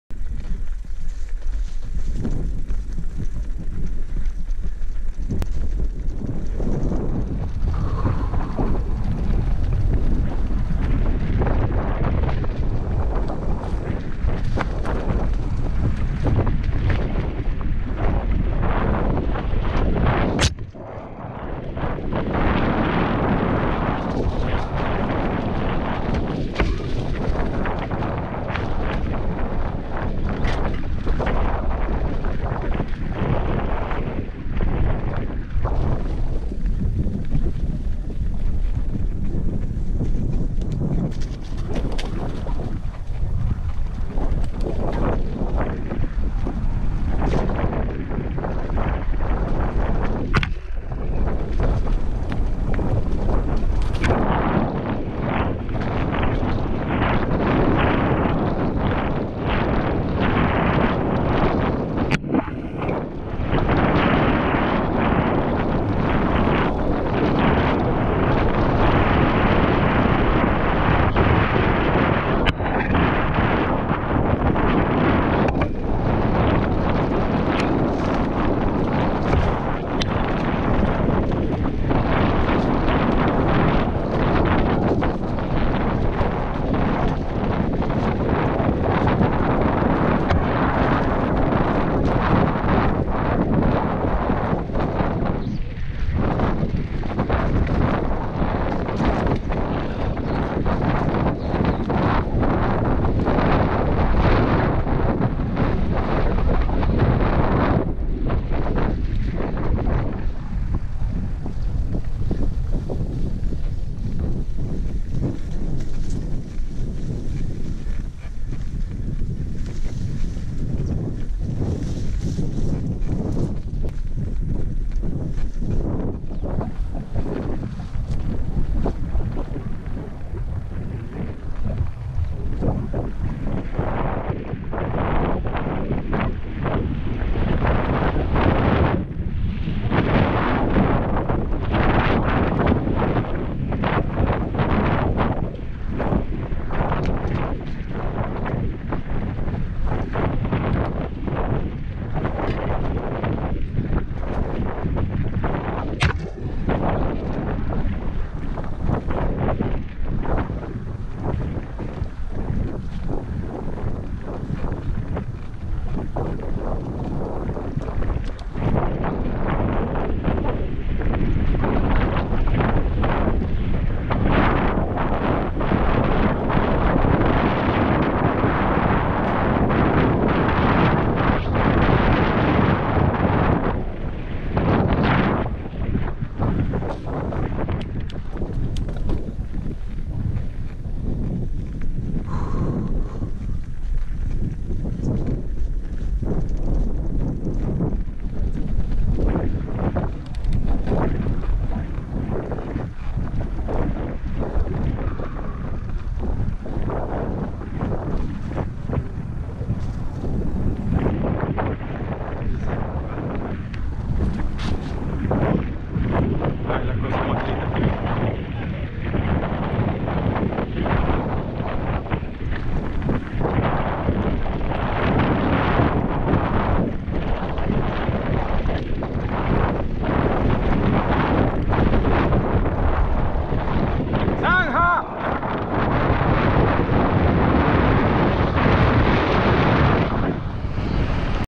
Trillo 4 - Competencia Punto a Punto B-Rider 2017
4to y último trillo largo, ya para llegar al final... esto fue de la competencia en Mayo 2017 denominada PUNTO A PUNTO - Dos Mangas Sinchal organizada por el local B-Rider de Guayaquil.